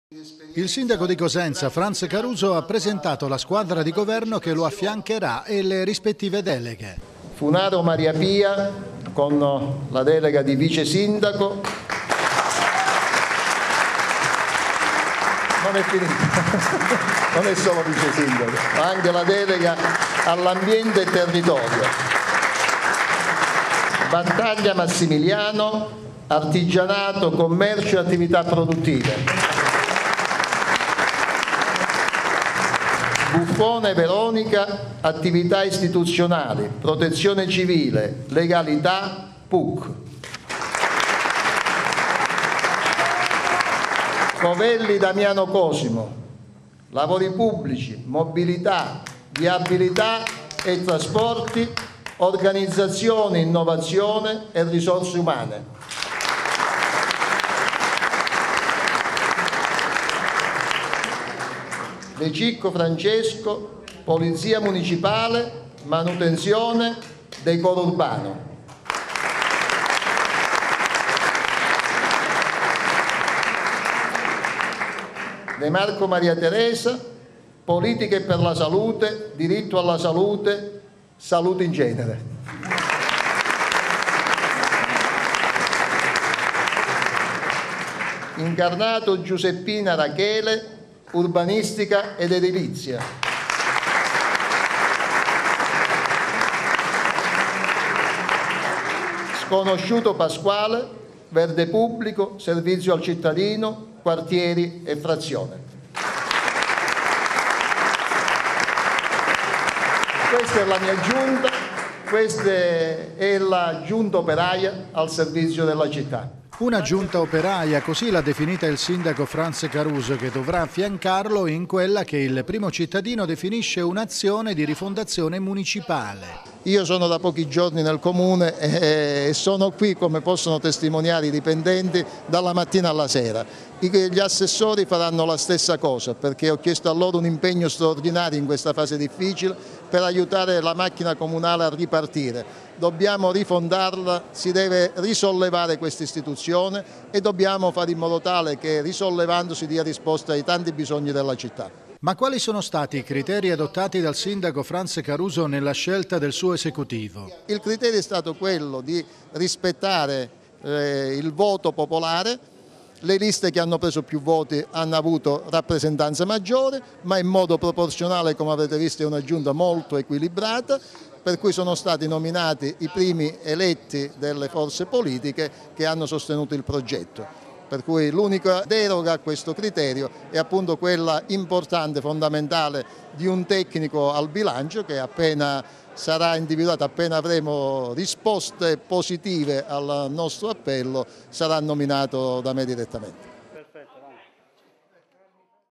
Cosenza: Comune, il sindaco presenta la sua Giunta
Il sindaco di Cosenza, Franz Caruso, ha presentato la squadra di governo che lo affiancherà e le rispettive deleghe. “Una Giunta operaia”, così l'ha definita il sindaco Franz Caruso, che dovrà affiancarlo in quella che il primo cittadino definisce “un'azione di rifondazione municipale”. Ma quali sono stati i criteri adottati dal sindaco Franz Caruso nella scelta del suo esecutivo?